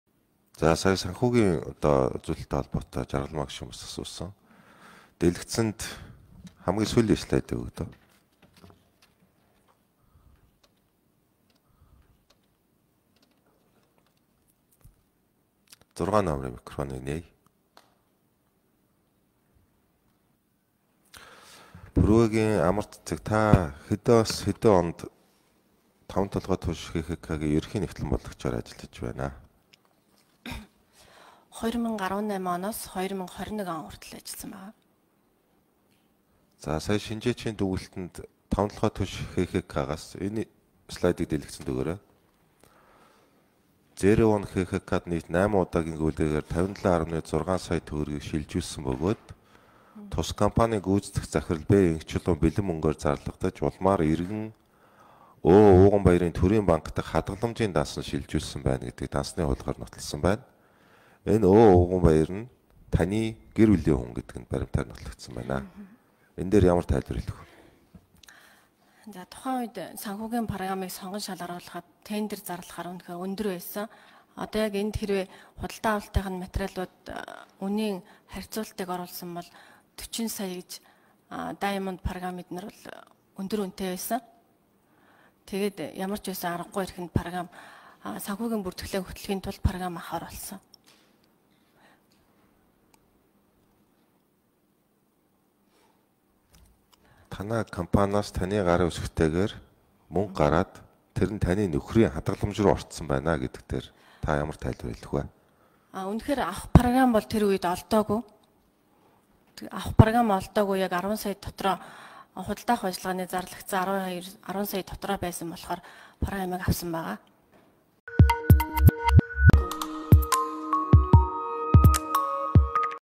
Б.Энхбаяр: Та нөхөр Ө.Ууганбаярын ХАДГАЛАМЖИЙН дансанд 57.6 сая төгрөгийг шилжүүлсэн байна.
Б.Энхбаяр: Та нөхөр Ө.Ууганбаярын ХАДГАЛАМЖИЙН дансанд 57.6 сая төгрөгийг шилжүүлсэн байна. Та үүнийг юу гэж тайлбарлах вэ?
П.Амарцэцэг: Үнэхээр тухайн үед авах программ олдоогүй...